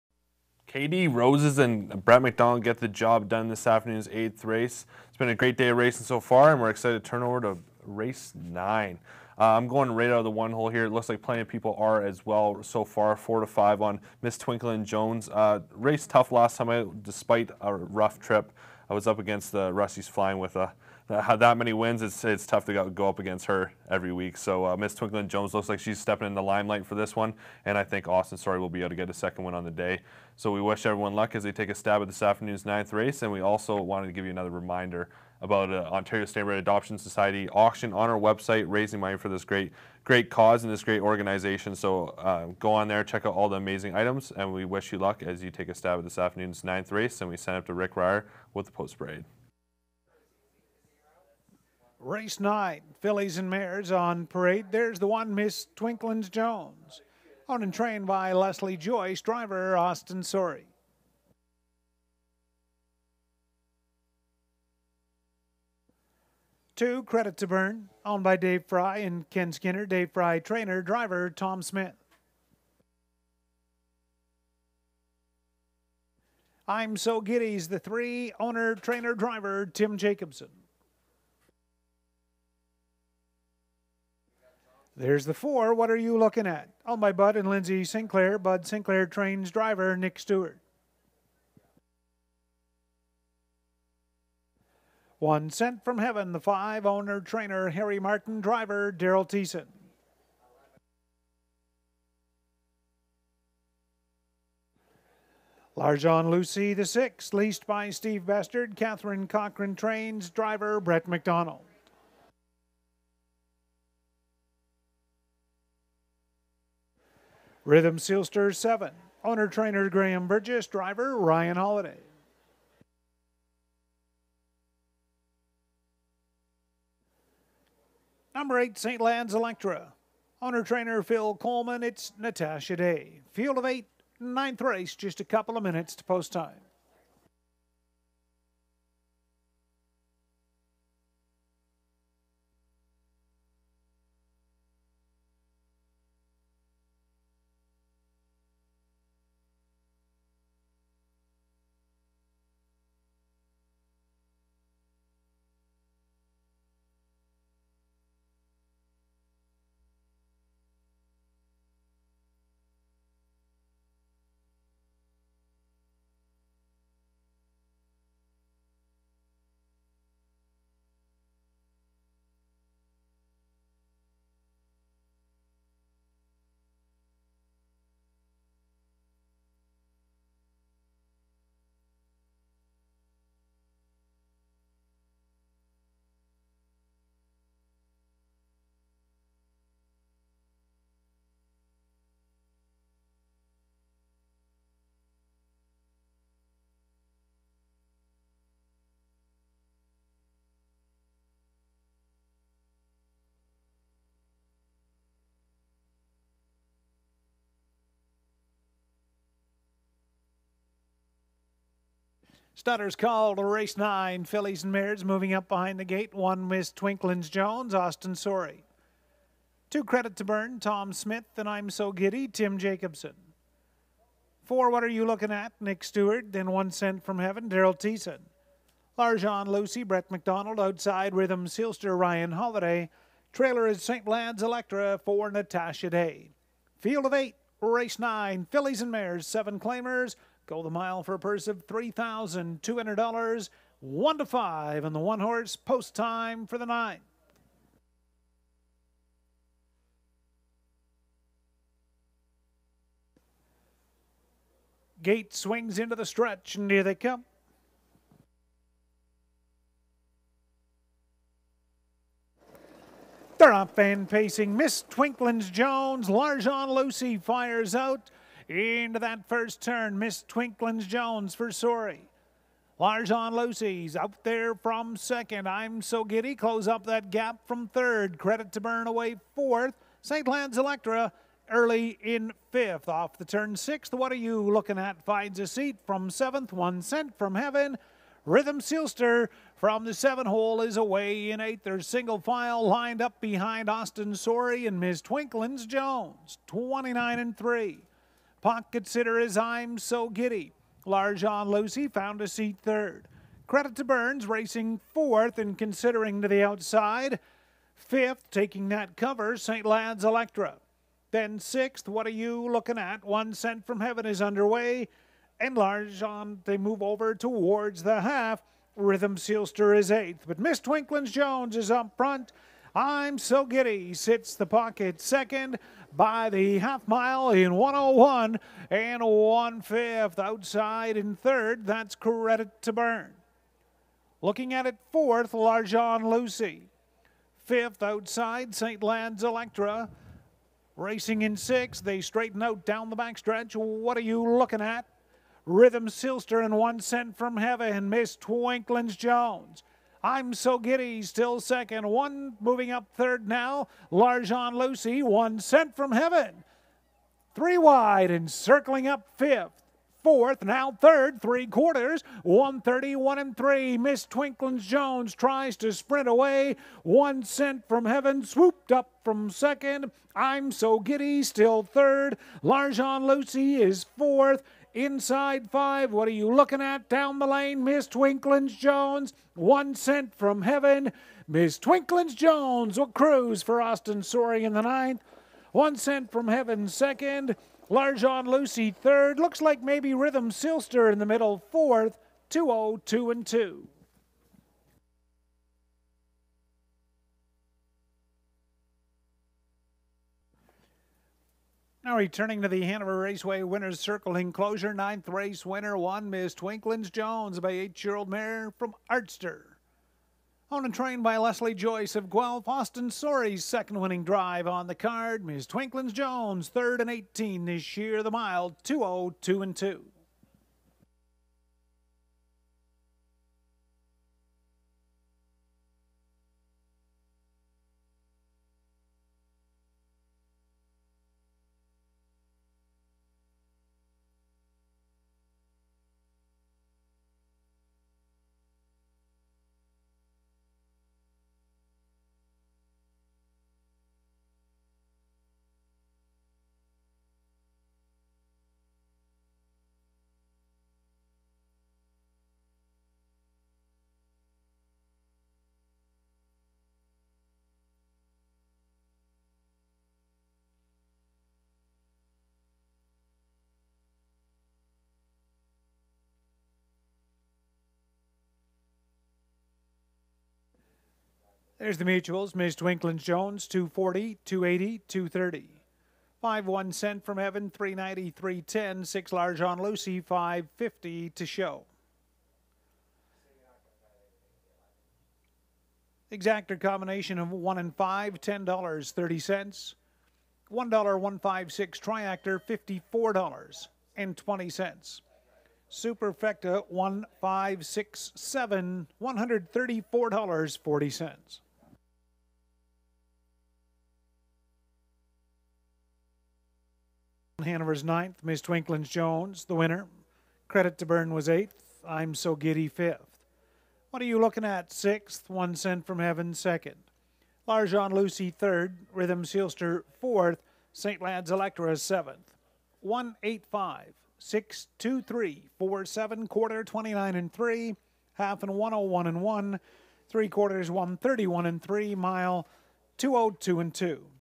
Hanover Raceway - Aug 29, 2020 - Race 9
1 - Misstwinklinsjones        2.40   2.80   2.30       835
5 - Onecentfromheaven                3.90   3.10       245
6 - Larjon Lucy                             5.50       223

$2 EXACTOR (1-5) paid 10.30, pool 806
$1 SUPERFECTA (1-5-6-7) paid 134.40, pool 809
$2 TRIACTOR (1-5-6) paid 54.20, pool 1151
.20$TR PAYS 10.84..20$SF PAYS 26.89

1 MILE, PACE, PURSE $3,200.
FILLIES & MARES - CLAIMING $7000. (W-ALL) - FOR N/W $1000. LAST 5 OR A RACE IN
2020